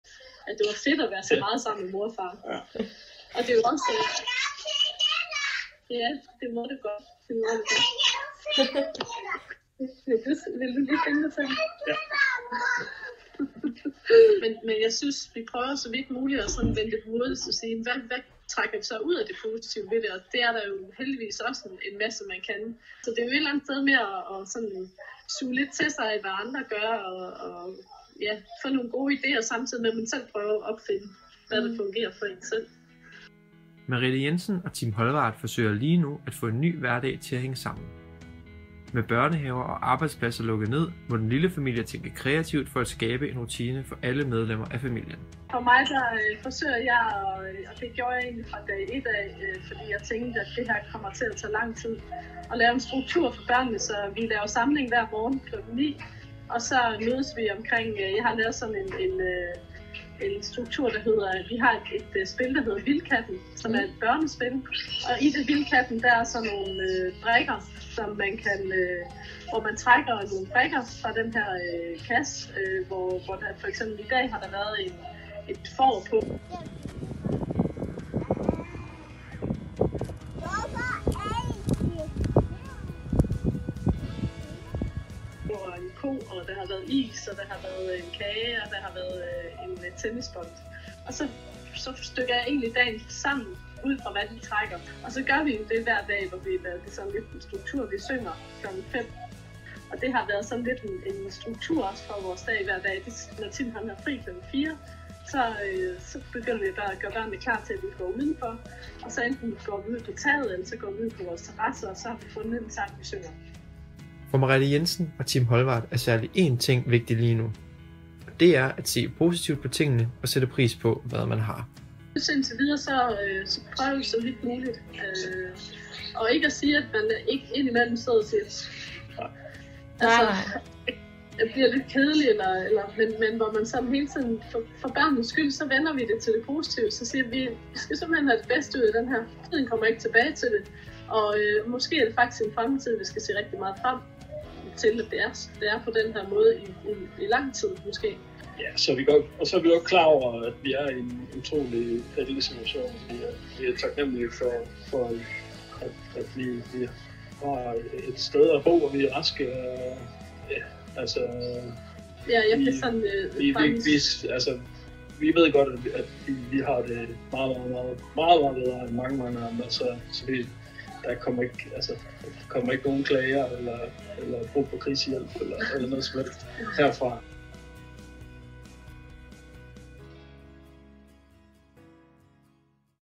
Familier må tænke kreativt i en tid uden børnehave og arbejde
Merete Jensen og Tim Holmvard forsøger i disse tider at skabe en hverdag for deres lille familie i Brabrand. Her er der brug for både kreativ og positiv tænkning, hvis alt skal gå op i en højere enhed.